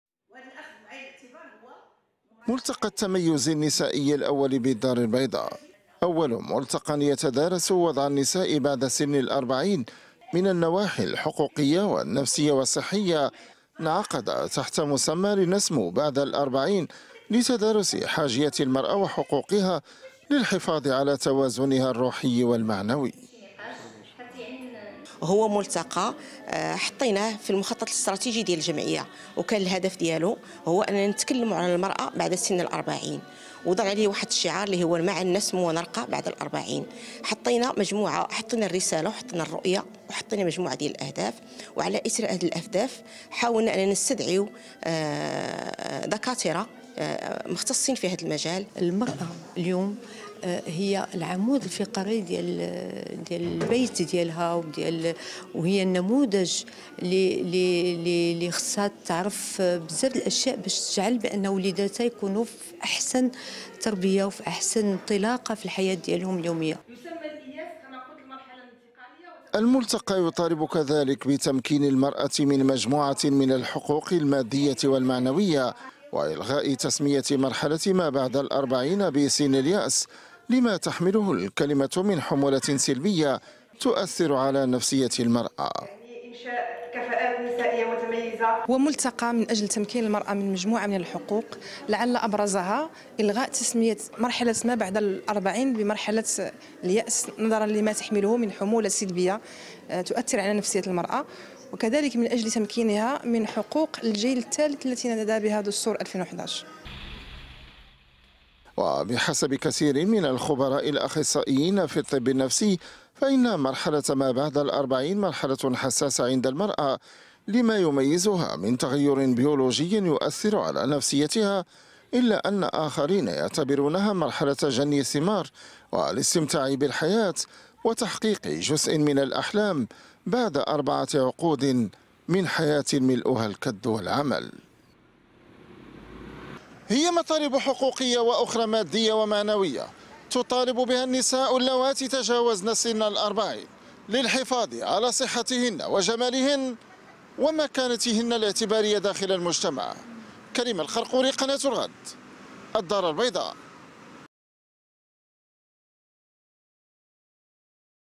إلغاء سن اليأس.. الدار البيضاء تستضيف ملتقى التميز النسائي الأول
انطلقت في المغرب فعاليات "ملتقى التميُز النسائي الأول" في مدينة الدار_البيضاء تحت شعار "لنسموا بعد الأربعين".. يسلط الملتقى الضوء على حقوق النساء في هذه السن.
المزيد من التفاصيل في سياق تقرير كريم قرقوري - مراسل الغد - الدار البيضاء